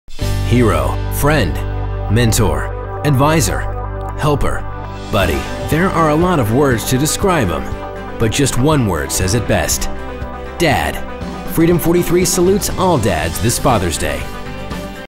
FATHER"S DAY (15 second spot)
This was made for Freedom 43 in Oklahoma City for Father's Day 2011. After Effects and Photoshop was used. I also made a 30 second spot as well.